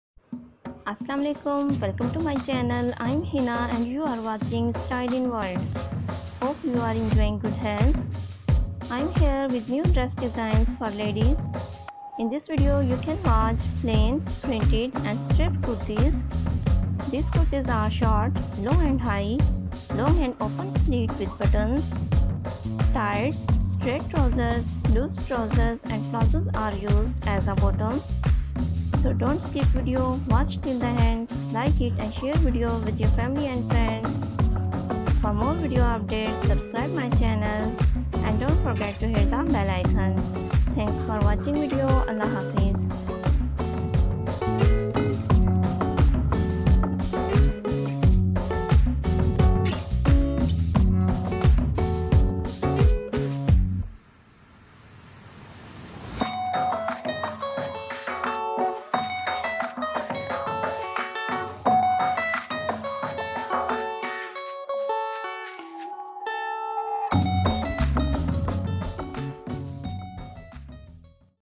Casual dresses for women............... unique designs
New stylish outfits designs for women
Plain, printed and stripped kurtis are displayed in this video
There are short, long, low and high kurtis with open slits and buttons
Tights, straight trousers, loose trouser, and plazoos are used as a bottom.